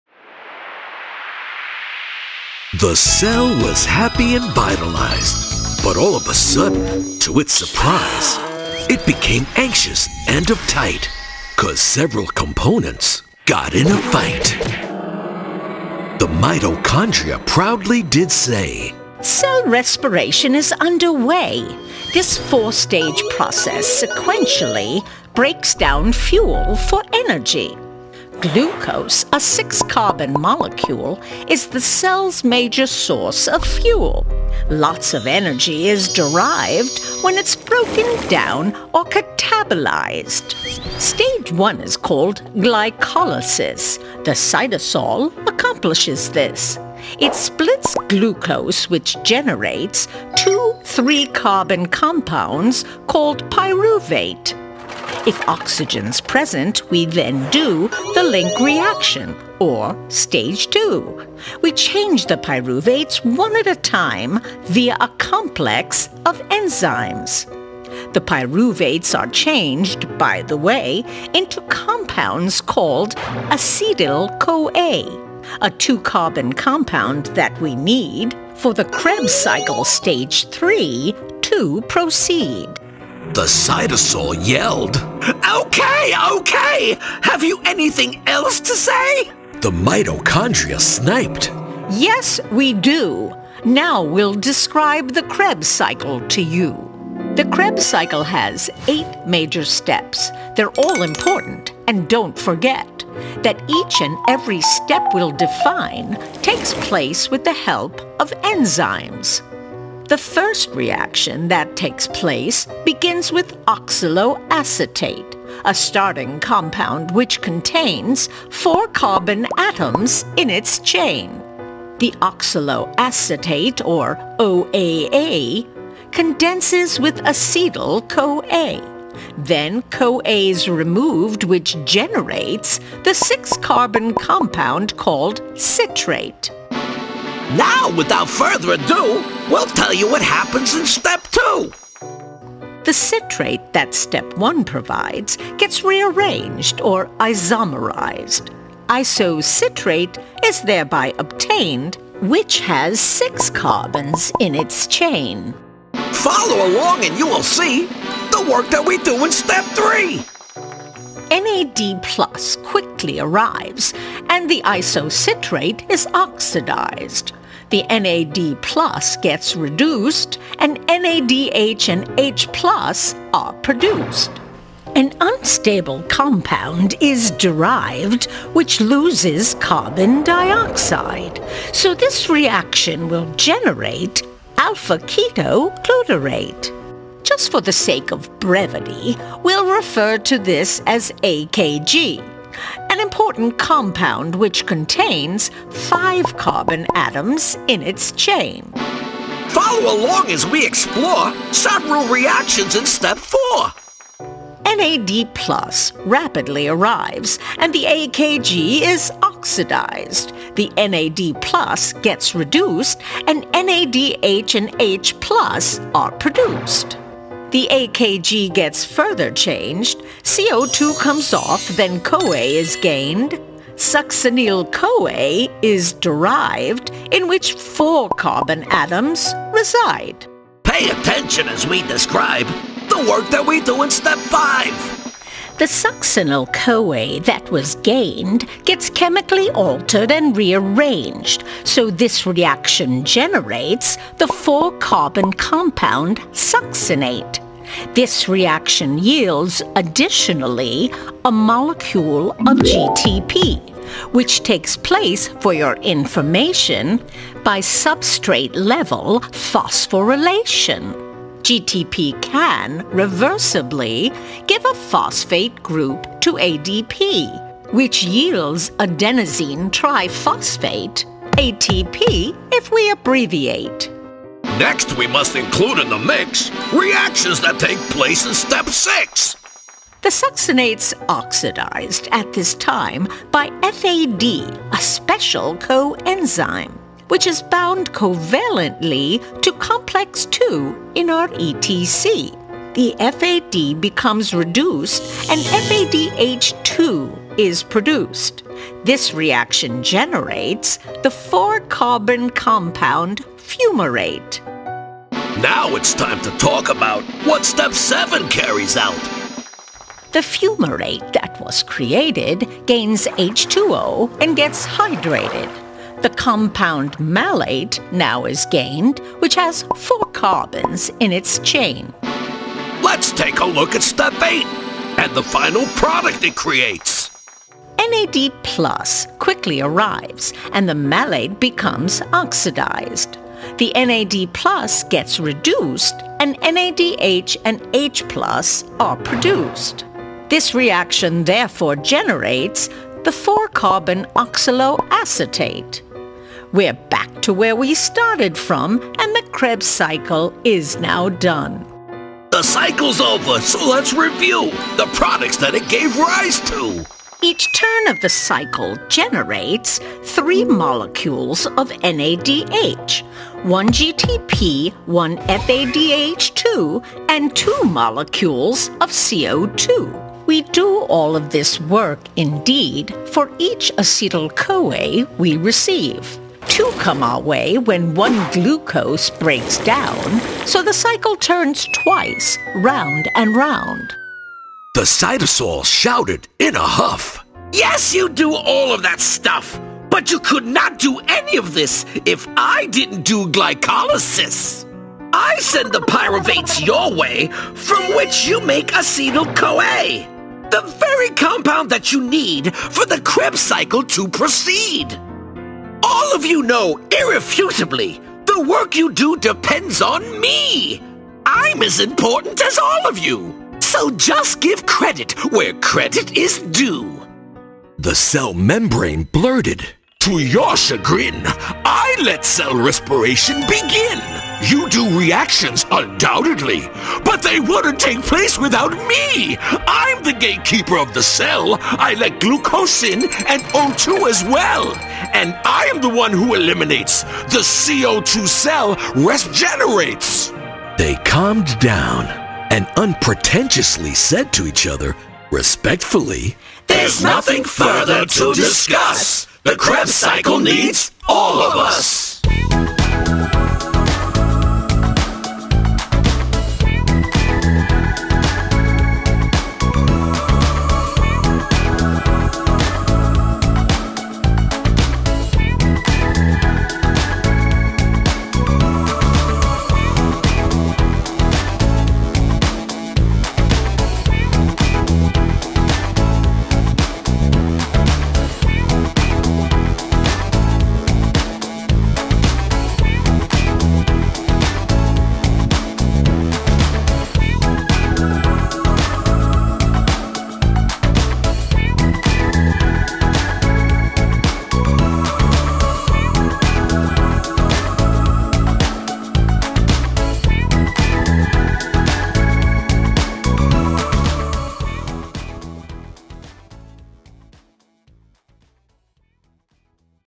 The Krebs Cycle Simplified | Animated Cartoon Video
The Krebs Cycle can be daunting. but not anymore! Our video describes this complex cycle in a fun, fast, and humorous way. Watch the mitochondria boast that they play the most important role in the cycle - see why the cytosol and cell membrane disagree! The rhyming dialogue, vivid animations, and dynamic sound effects bring this otherwise invisible cycle to life! Suitable for high school and higher ed students.